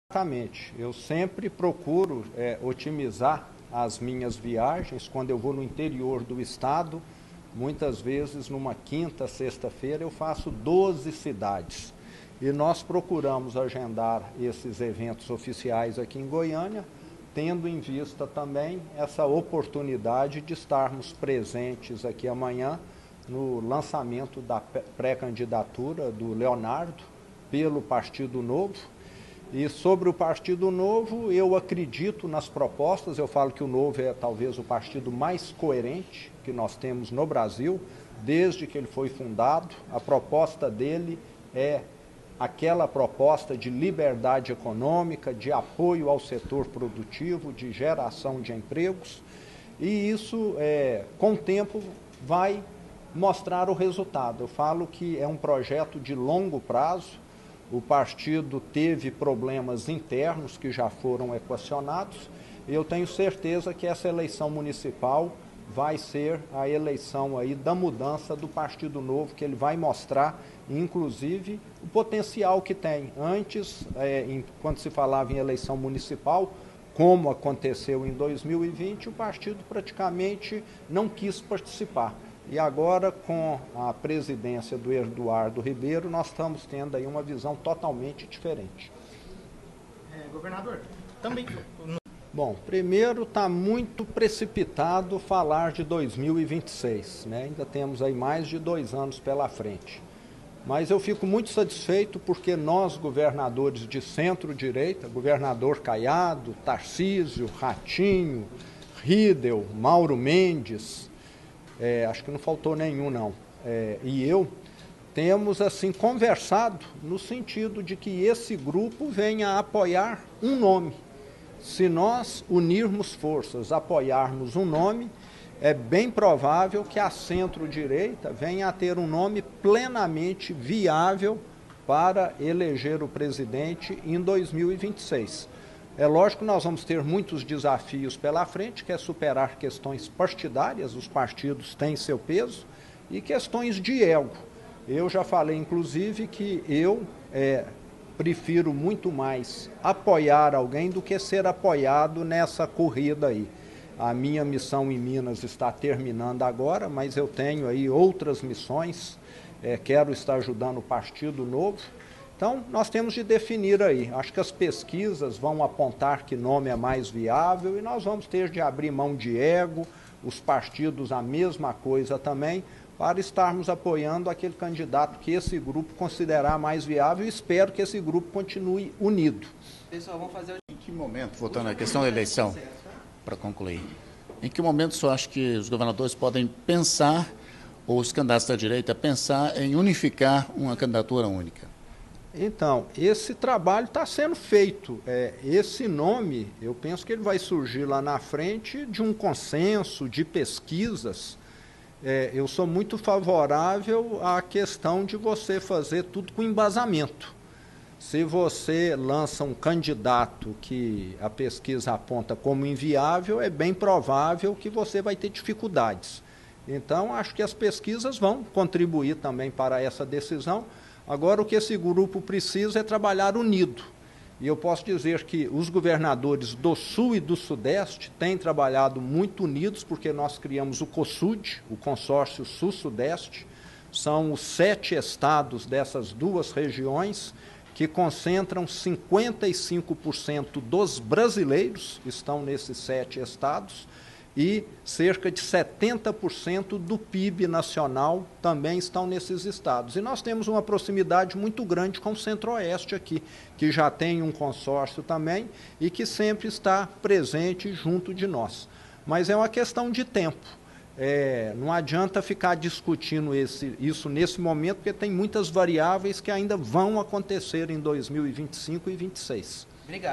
Em Goiânia, Zema defende união de governadores da centro-direita para 2026, “sem egos”
Pesquisas e consenso, “sem egos”, devem apontar o nome mais viável para disputar a presidência da República em 2026 representando governadores e partidos de centro-direita. A opinião é do governador de Minas Gerais, Romeu Zema, que chegou nesta sexta-feira (7) em Goiânia para agenda partidária e de visitas.

No sábado, como mostra reportagem do Diário de Goiás, Zema participa de um encontro estadual do Novo. Nessa sexta, ele esteve na Associação Comercial de Anápolis. Também almoçou na Federação da Agricultura (Faeg) e a tarde iria a evento da Associação Comercial e Industrial de Goiás (Acieg).

Link na bio 👉 Últimas notícias